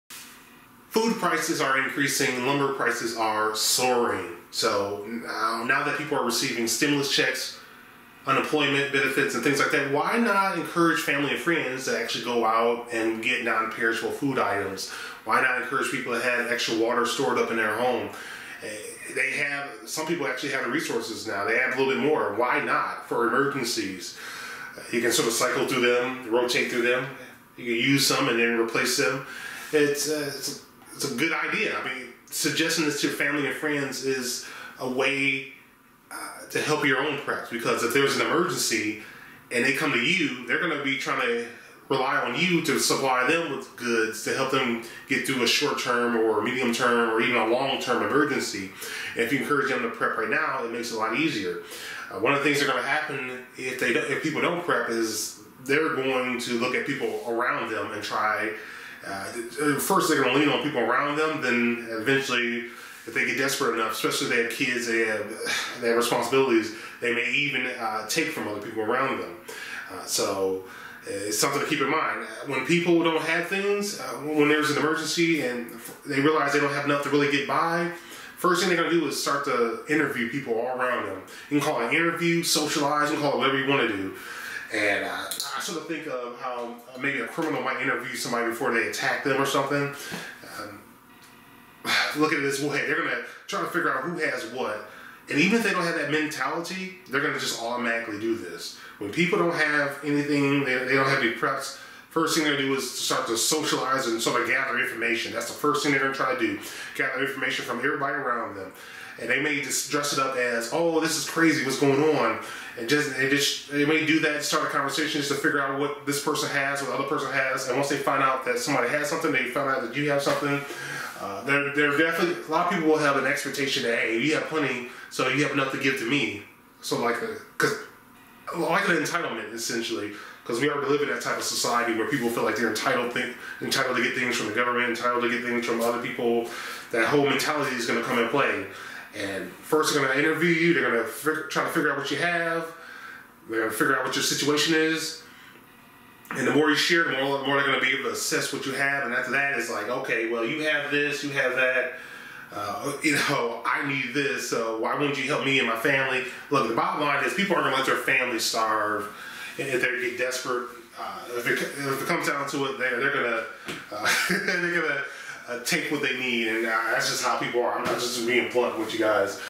The First Thing Unprepared People Will Do When The SHTF
If there is a serious crisis, this will be the first thing that unprepared people will do.